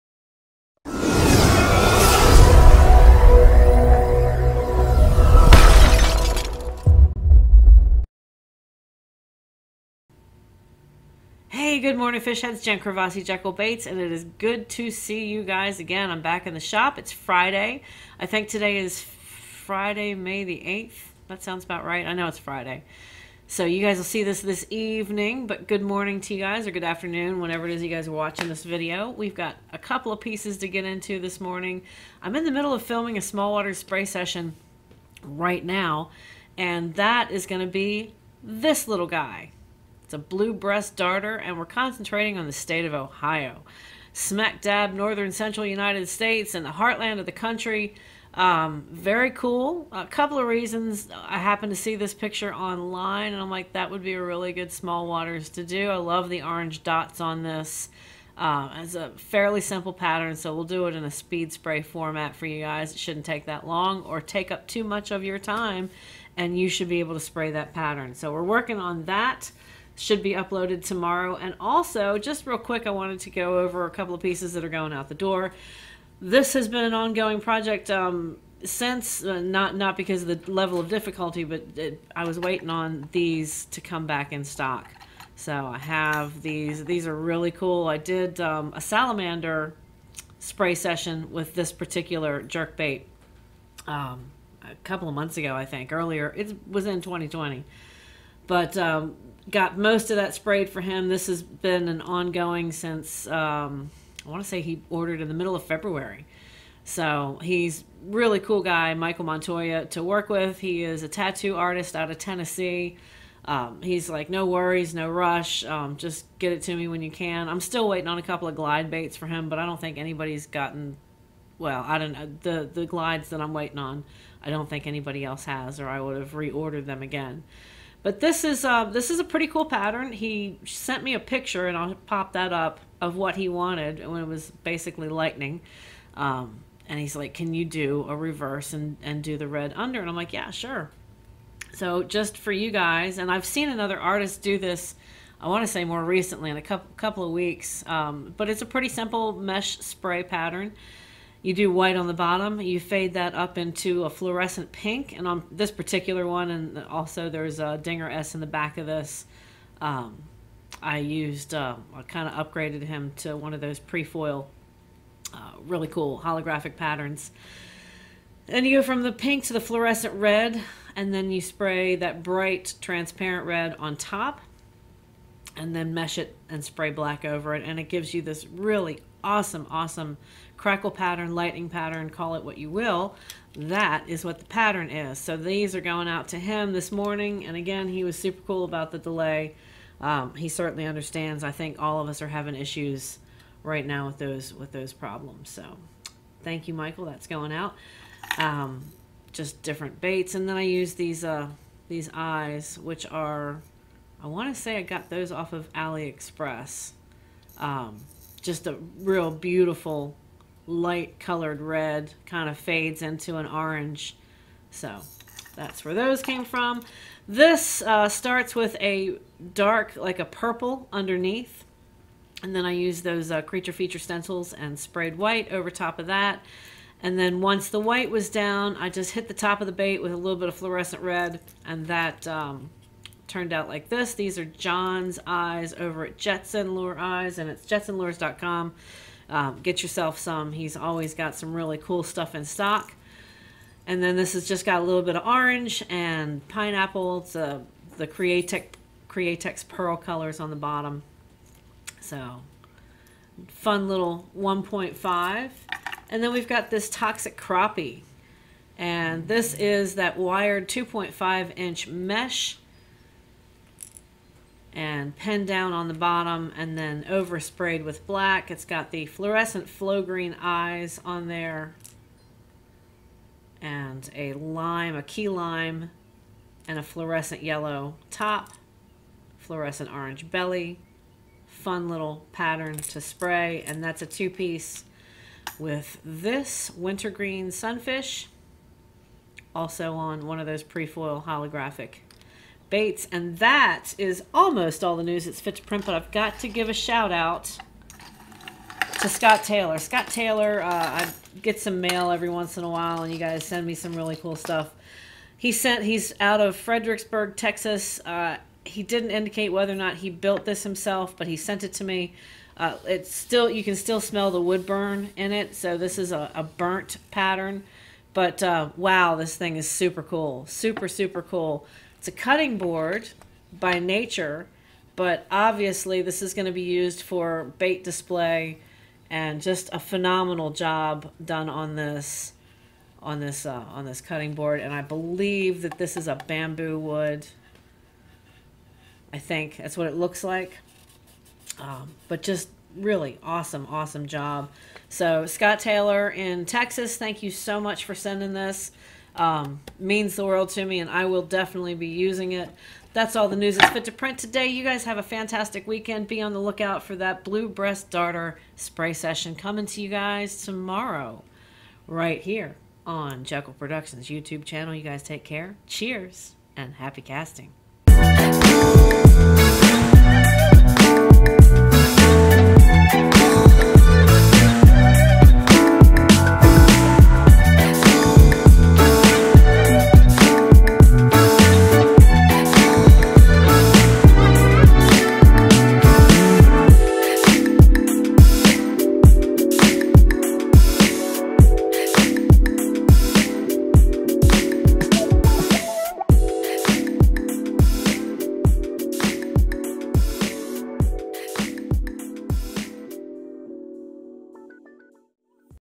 Friday Night Shop Update - 05/08/2020 (Red Lightning, Toxic Crappie and Mail Call Shout Out)
Featuring Mike Montoya's 10 piece set, Jettson Lure Eyes and a very cool gift from a subscriber! Thanks for stopping by. Have a great day.
Cheers, Jen at JBCo.

Respirators:
3M (Large)
3M (Small)